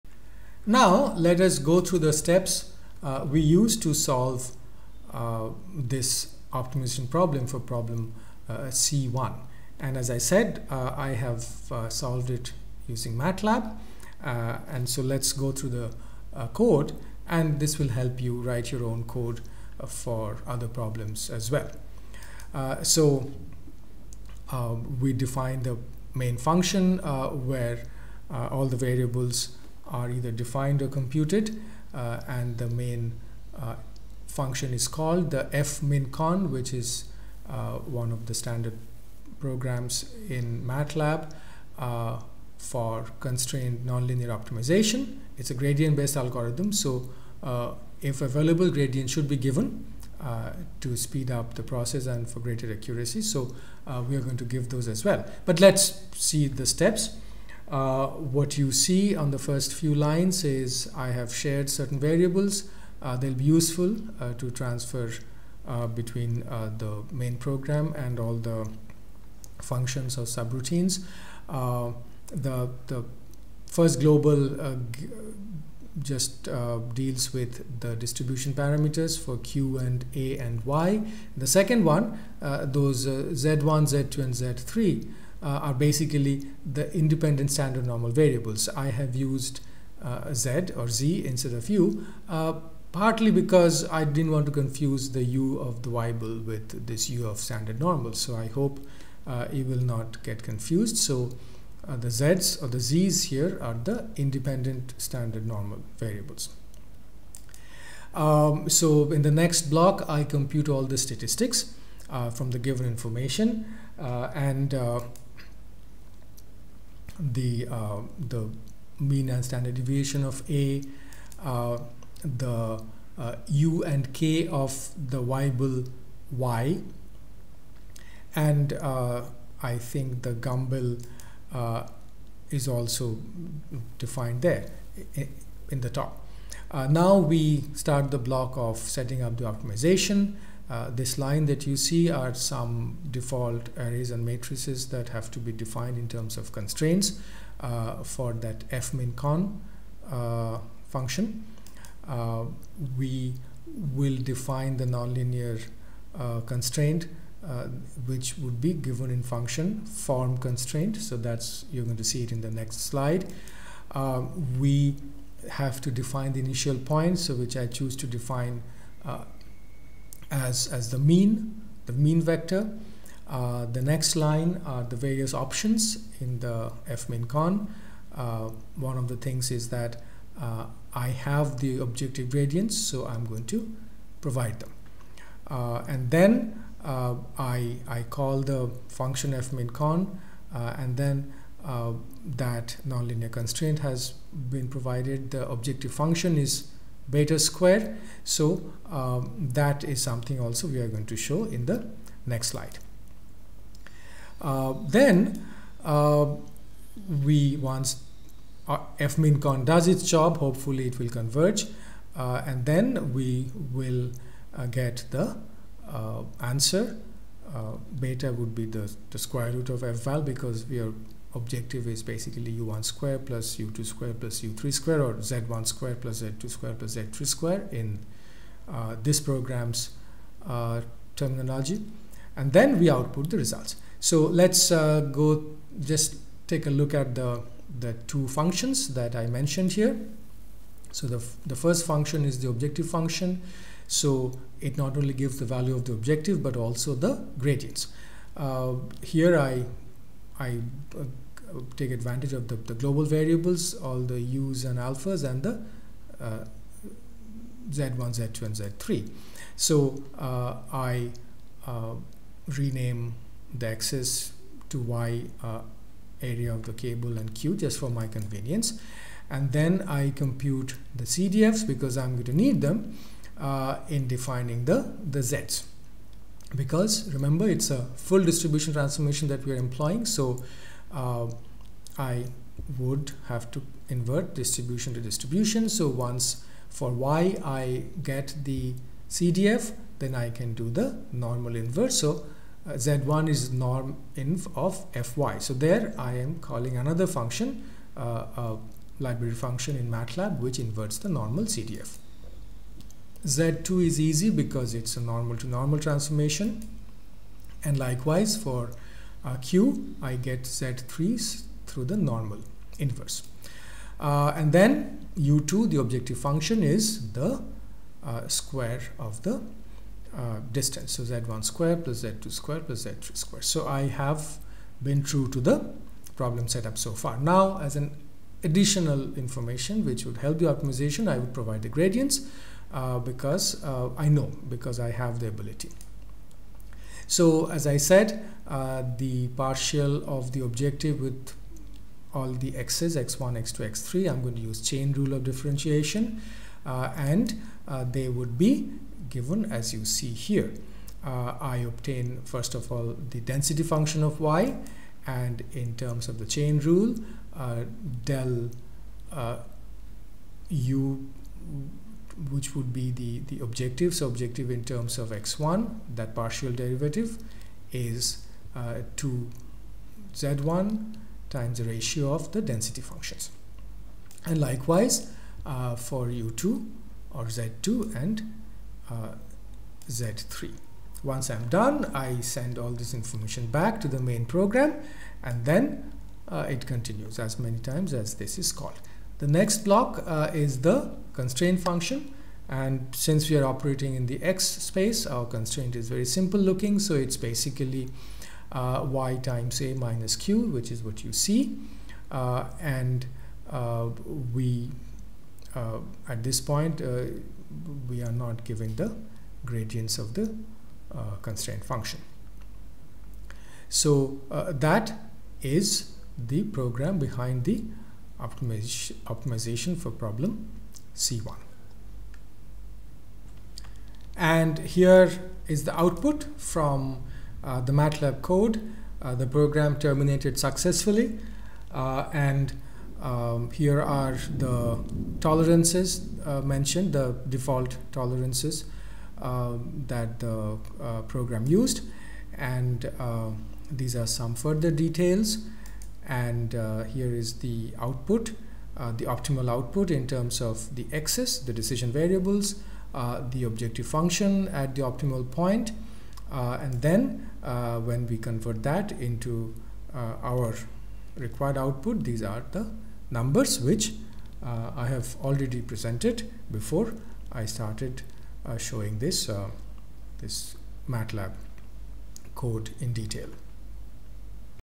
STRUCTURAL RELIABILITY Lecture 22 module 03: First order reliability methods (FORM) - examples
FORM Example C1 (contd.) Matlab Code and explanation